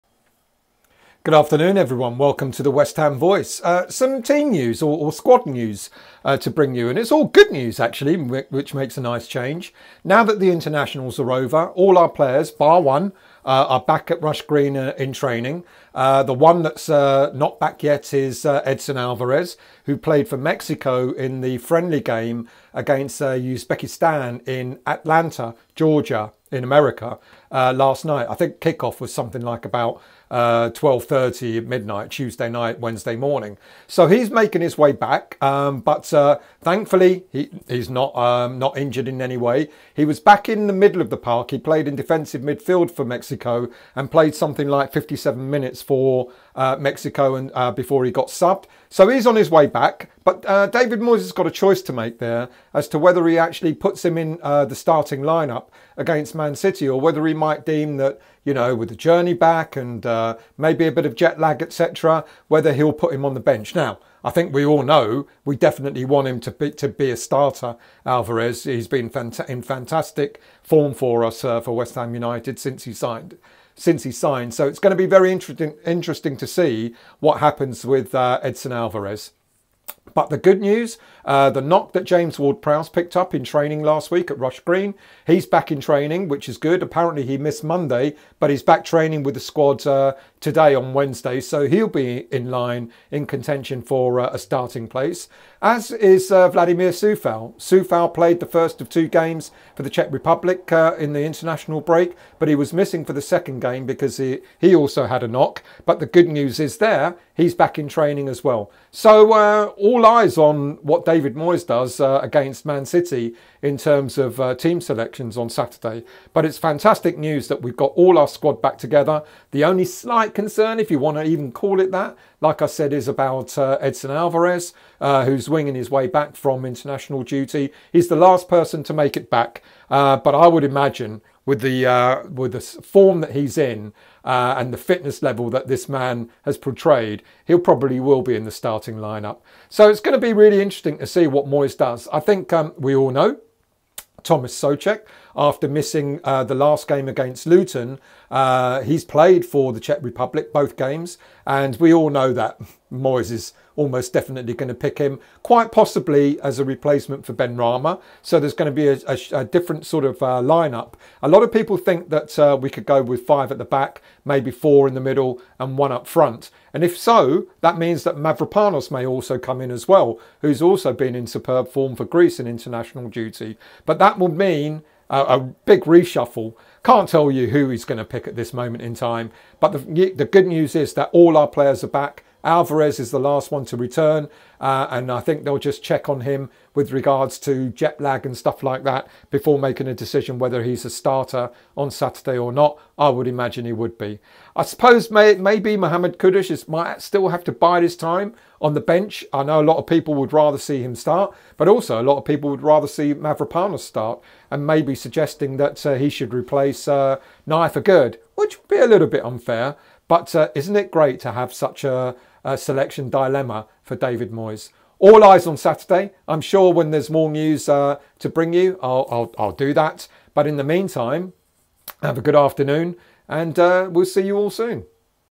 LATEST SQUAD NEWS UPDATE | ALVAREZ, WARD-PROWSE AND COUFAL
David Moyes will have his work cut out for him when it comes to team selection against Man City on Saturday.

All players are back from international duty and Moyes has a full squad to select from

Who will be in his starting eleven?